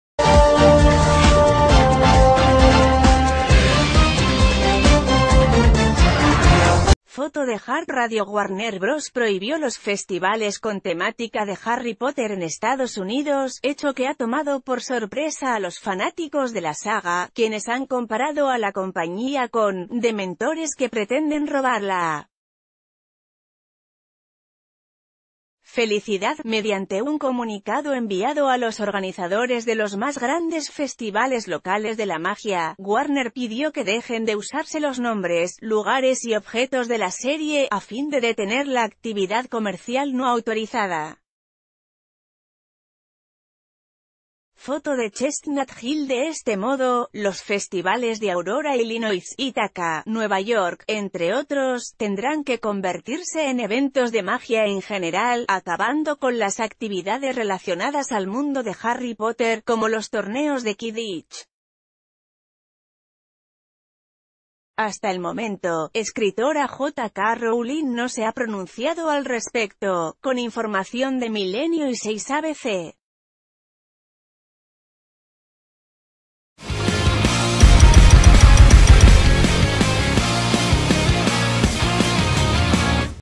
Warner Bros prohíbe festivales de Harry Potter
Foto de Heart RadioWarner Bros prohibió los festivales con temática de Harry Potter en Estados Unidos, hecho que ha tomado por sorpresa a los fanáticos de la saga, quienes han comparado a la compañía con “dementores que pretenden robar la felicidad”.Mediante un comunicado enviado a los organizadores...